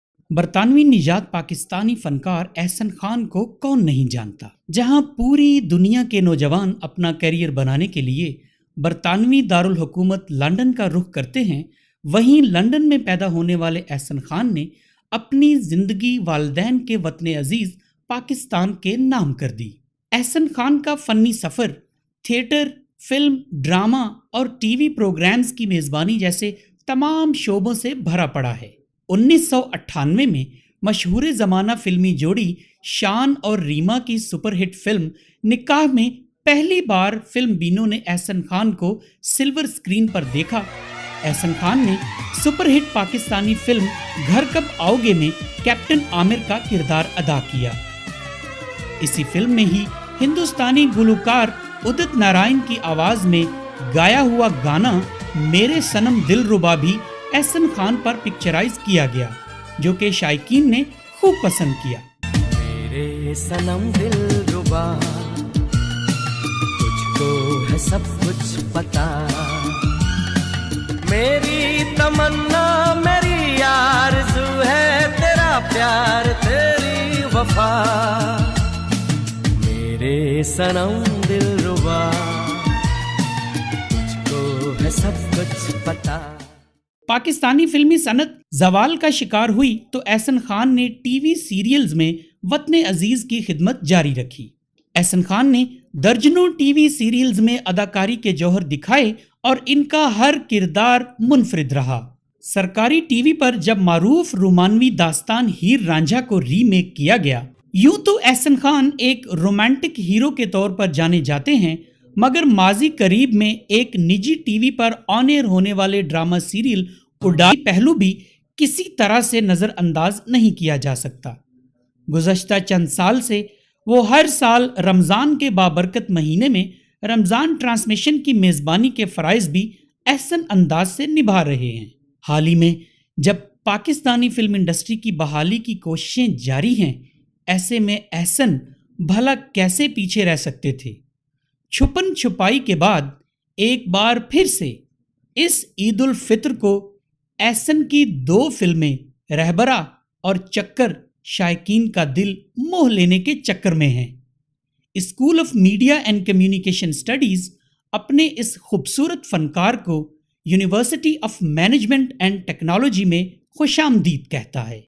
Ahsan Khan, British Asian OR Pakistani Actor | Documentary/Package | 25 Years of his Showbiz Career
Title: Ahsan Khan, British Asian OR Pakistani Actor | Documentary/Package | 25 Years of his Showbiz Career

Video Type: A Documentary/Package

Objective: This video is an introductory video on Showbiz star Ahsan Khan who was invited as guest at University of Management and Technology Lahore.

Description: Ahsan Khan is a British Asian Pakistani film, theatre, drama actor and television host. He's worked in film, TV, theatre in last 25 years. This short video describes his career lasting 3 decades.